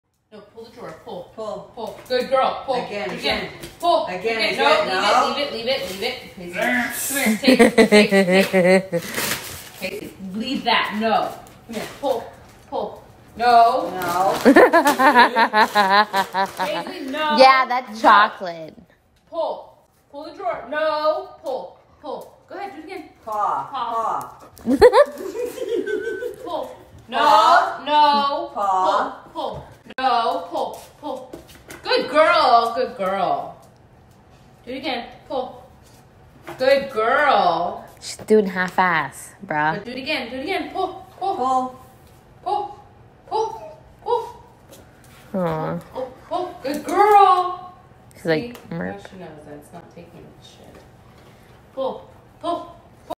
Dog learns how to open a kitchen drawer! | English Cream Golden Retriever
Hi. My name is Paisley! I do a lot of weird and entertaining tricks. In this video, my mommy teaches me how to get in a kitchen drawer, little does she know, this will definitely come in handy when she’s not around…👀🍕🍪

White Golden Retriever